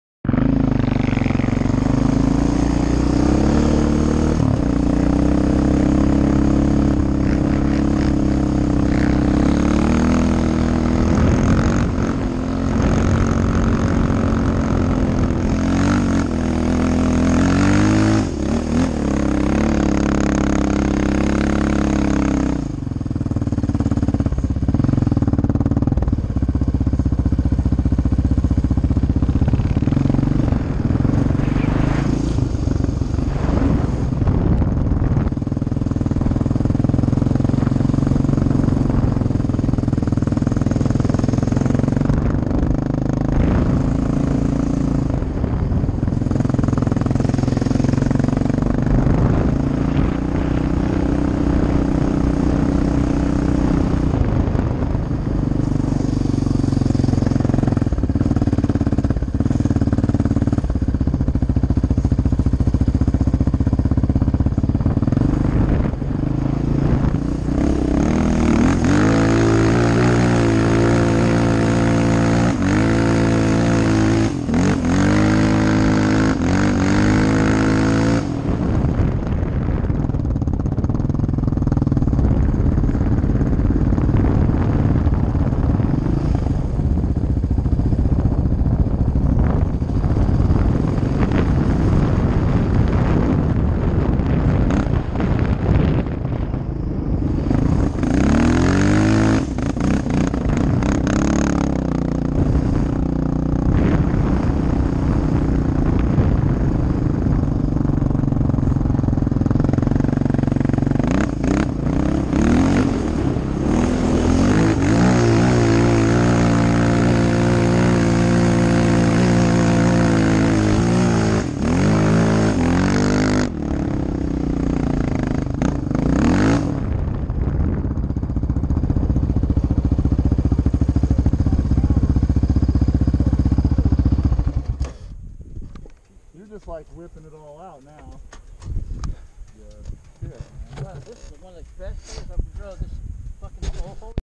Christmas day at winchester bay Oregon dunes 2016
Raptorguy with David and Eric riding the dunes at winchester bay Oregon dunes